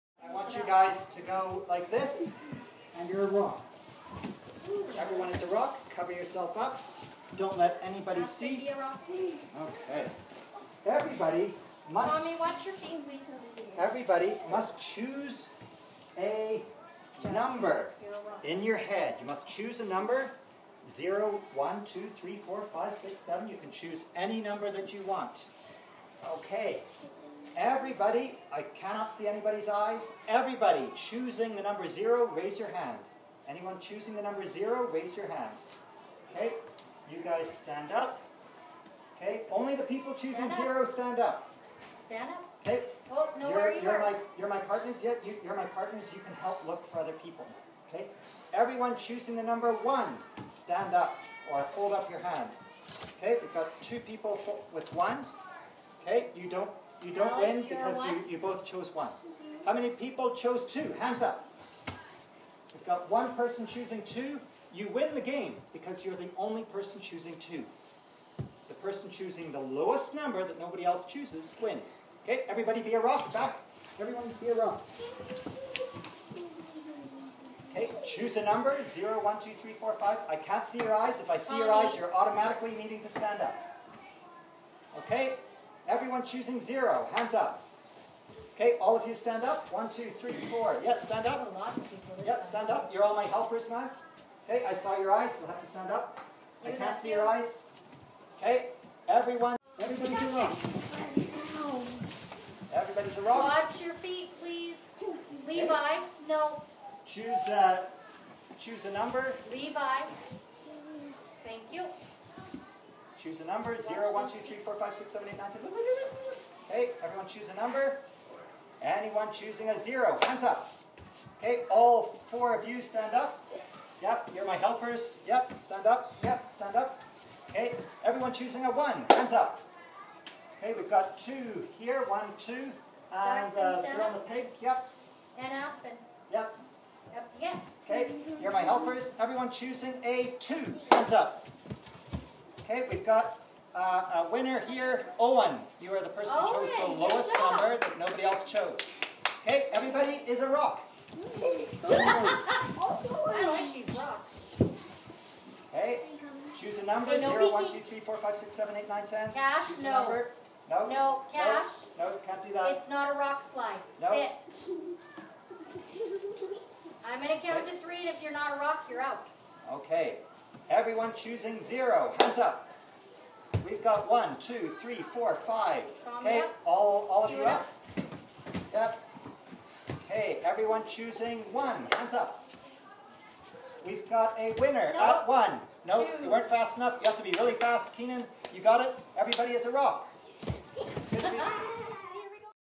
Rock - Low Unique Number Game
This is the best MathPickle filler game for grades K-2. If you need five minutes of energetic math - try it. Each game takes about 45 seconds.  This is part of the key to its success. Children who have not learned the art of losing are quickly thrown into another game before they have a chance to get sad. Try plotting the winning number. What is a winning strategy? Does the winning strategy depend on the number of children playing?

Rules:
1) Everyone makes a rock shape with eyes closed.
2) Everyone chooses a number 0, 1, 2, 3, 4, 5, 6, 7, 8, ...
3) Teacher starts by calling out numbers consecutively starting at 0.
4) When a student hears their number being called they immediately raise a hand.
5) If more than one hand was raised - those students lose. They become your helpers.
6) If only one hand was raised that child wins.